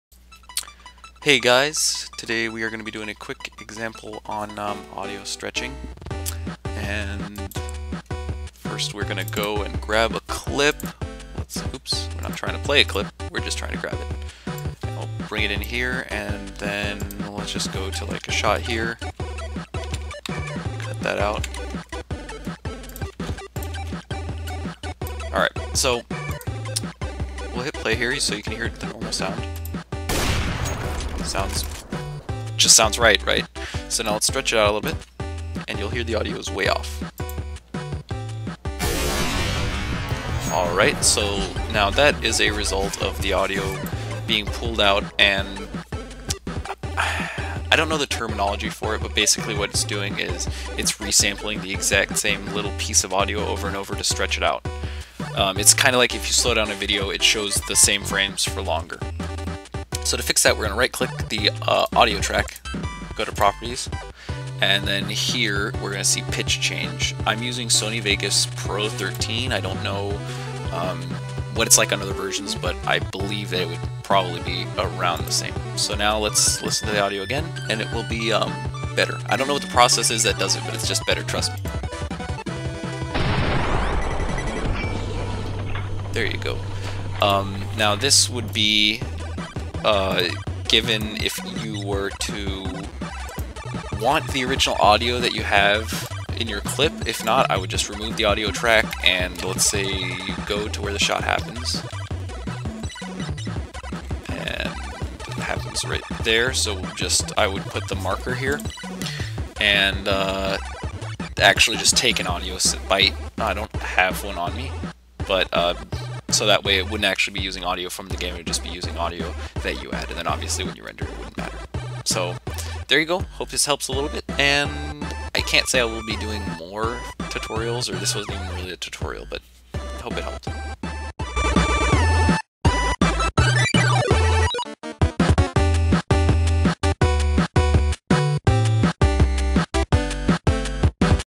Stretching Audio Example & Tutorial (Sony Vegas)
All content featured in this video was used with permission.

A tutorial and example of how stretching audio in a slowed video clip can increase quality.